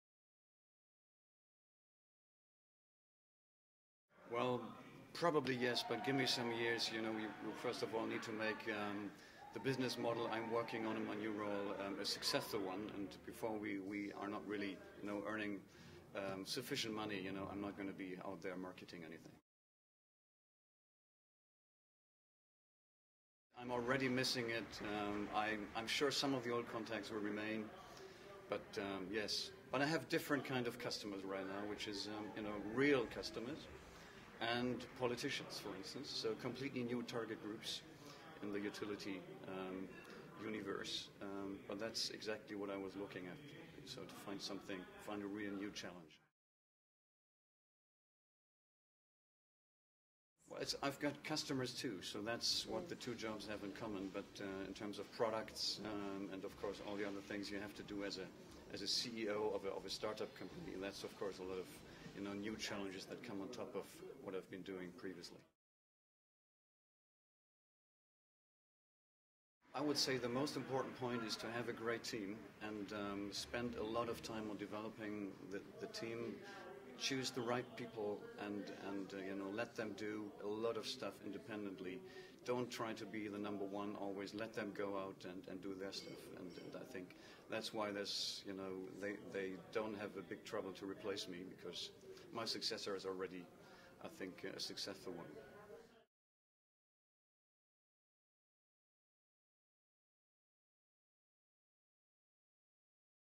RWE's Ingo Alpheus looks back on his time in IR
After spending 10 years as head of IR at German power company RWE, it was fitting that Ingo Alpheus, in year he stepped down from the role, was awarded best IRO at a large cap at the IR Magazine Europe Awards 2010. Alpeus goes on to become RWE's chairman of the management board.

Speaking at the awards, Alpheus advises other IR chiefs to focus on developing their team. 'Choose the right people and let them do a lot of stuff independently,' he tells IR magazine. 'Don't try to be the number one always.'